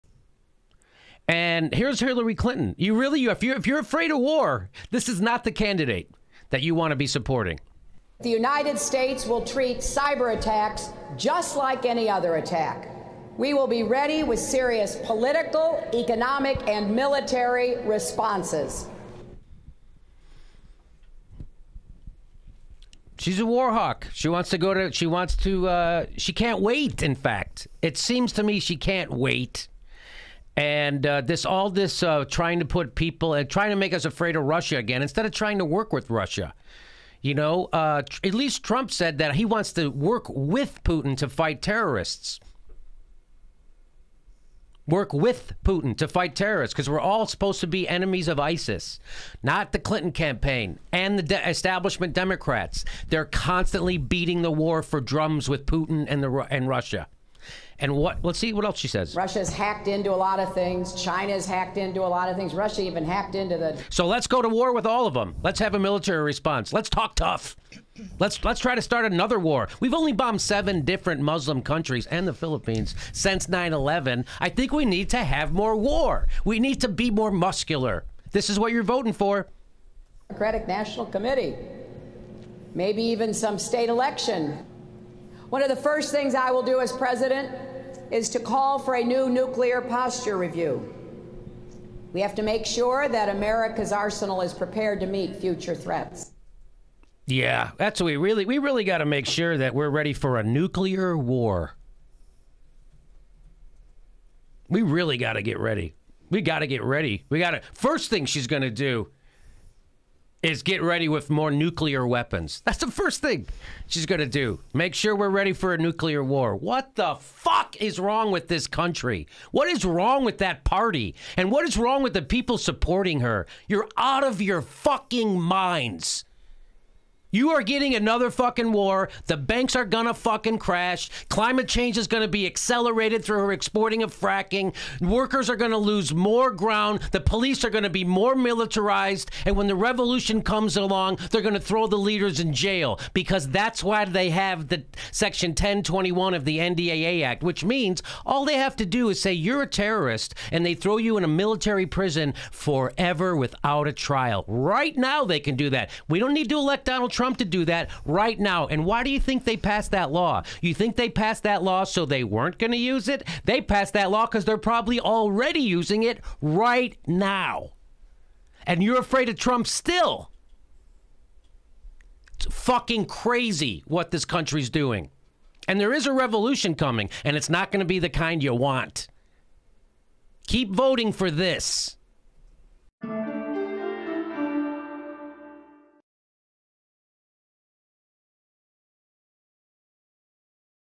Just Curious, What Do Hillary Supporters Think of THIS?
The 1st thing Hillary will do is pay back her Military Industrial Complex benefactors and push for new nuclear weapons.

Really?  You couldn't think of anything perhaps MORE important?  Or LESS Crazy??!!

Don't be a War Wanker, vote Dr. Jill Stein of the GREEN PARTY.